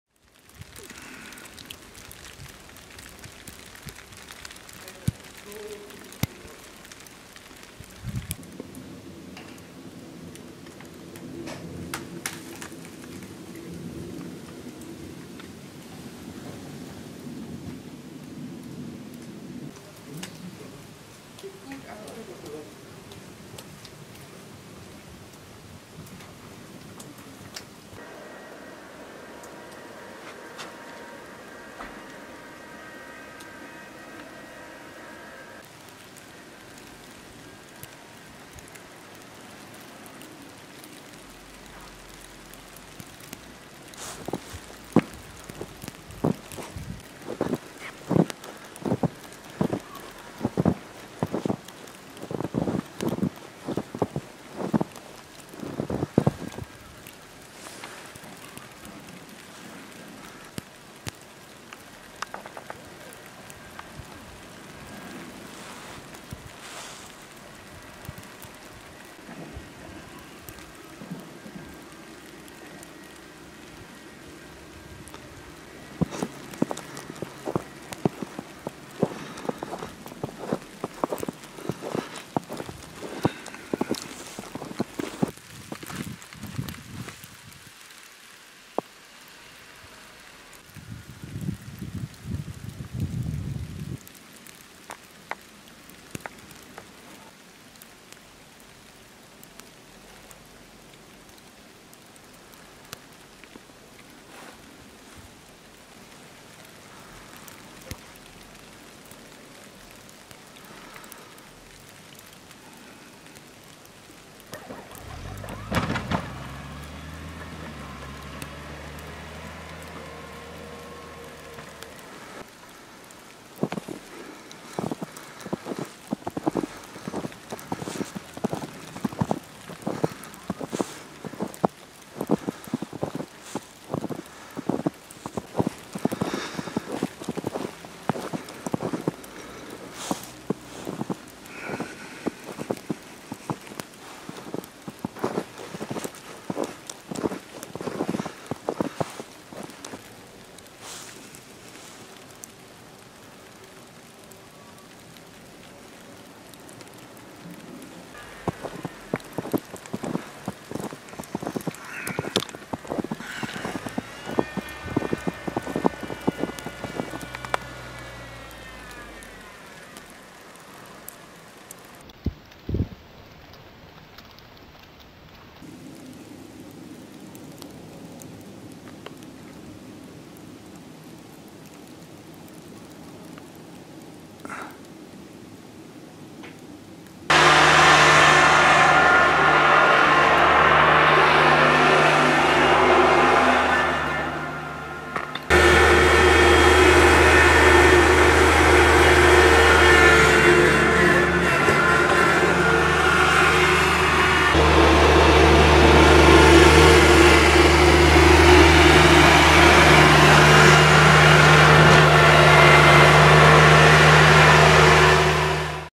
Schneechaos in München am 02.12.2023
Schneechaos in München! ❄️❄️❄️☃️ Mittlerweile liegen hier am nordwestlichen Stadtrand 50 cm Schnee! Die Straßen sind teilweise unpassierbar! Autos sind unter der dicken Schneedecke verschwunden. Der öffentliche Verkehr wurde eingestellt. Und es schneit noch kräftig weiter. Leider gibt es auch teils starken Schneebruch. Hier mein Video von 06:30 Uhr, bei mir vorm Haus aufgenommen. Quelle